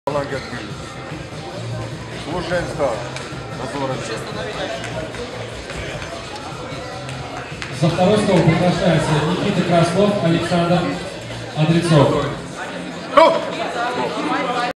Костя ФЕД (VEGA-Olympia 950) - Left Hand 001
XXI Первенство Москвы среди Юниоров-18
весовая категория +80кг (Левая рука) /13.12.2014/
ArmWrestling Club "VEGA-Olympia 950"